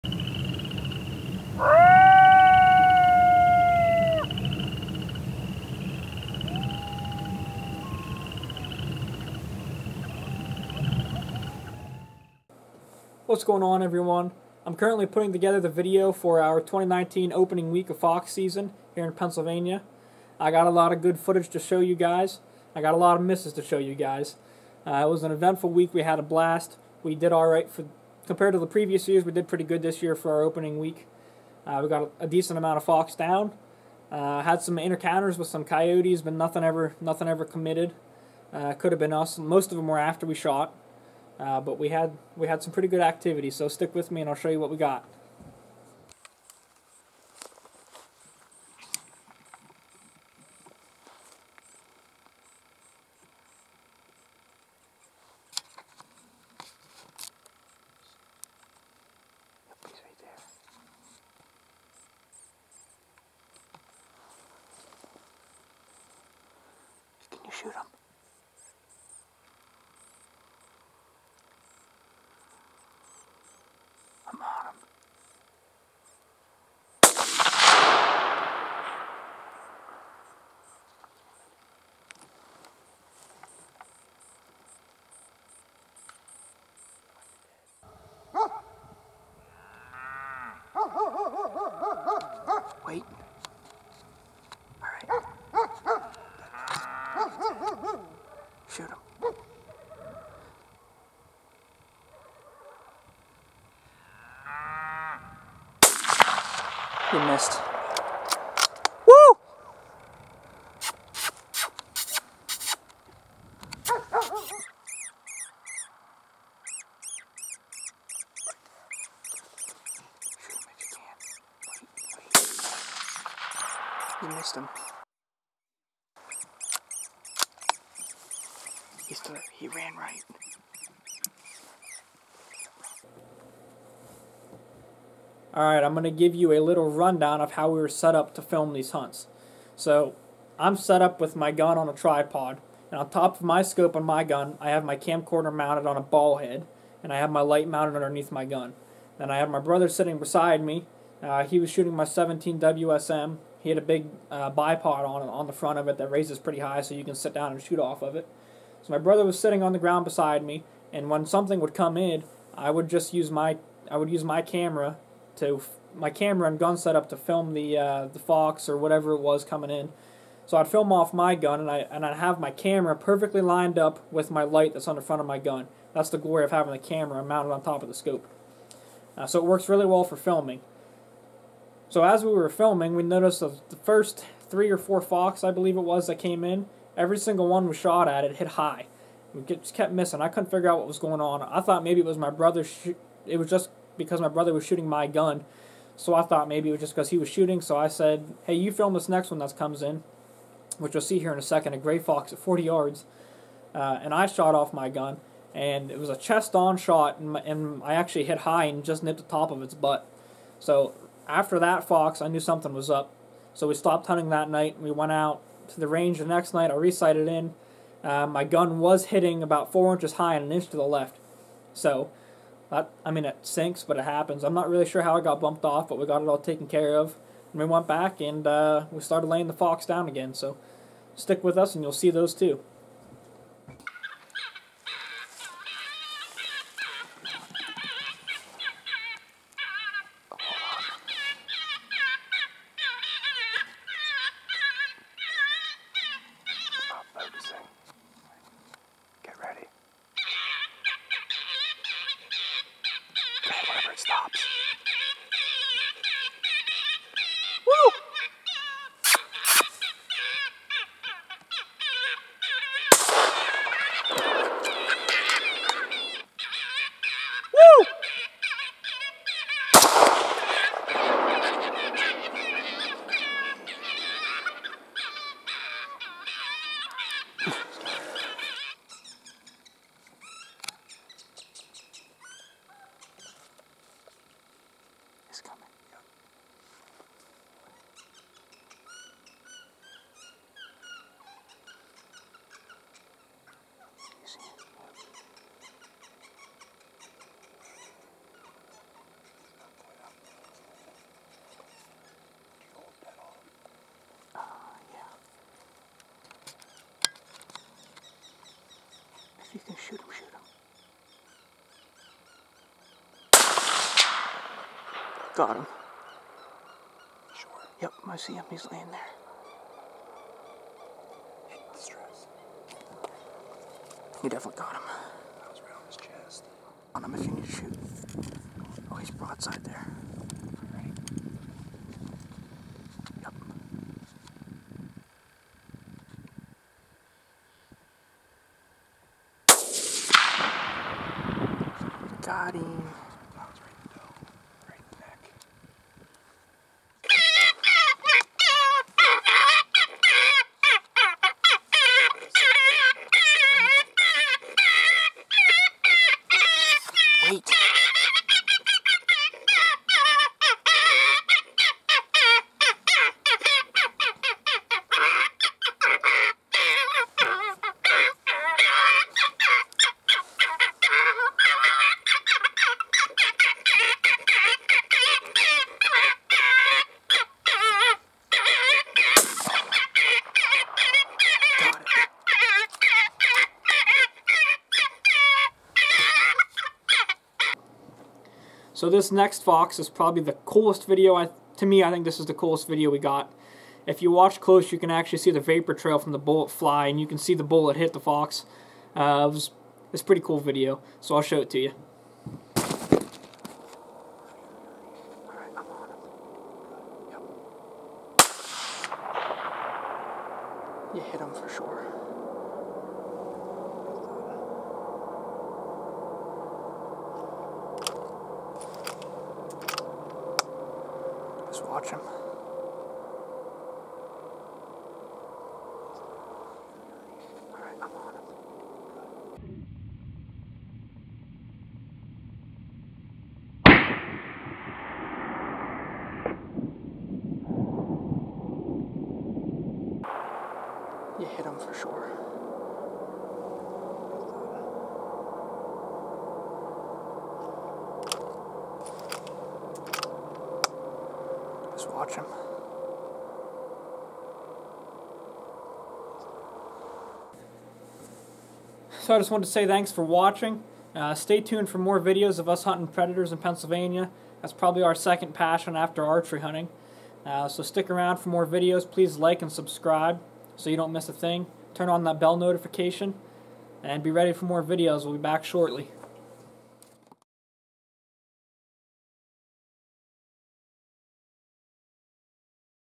2019 Pennsylvania Fox Opener | PA Predator Hunting | 17WSM | Coyote Cannon
Follow along as Ethan and Logan Stroup predator hunt during the opening week of the 2019 PA fox season.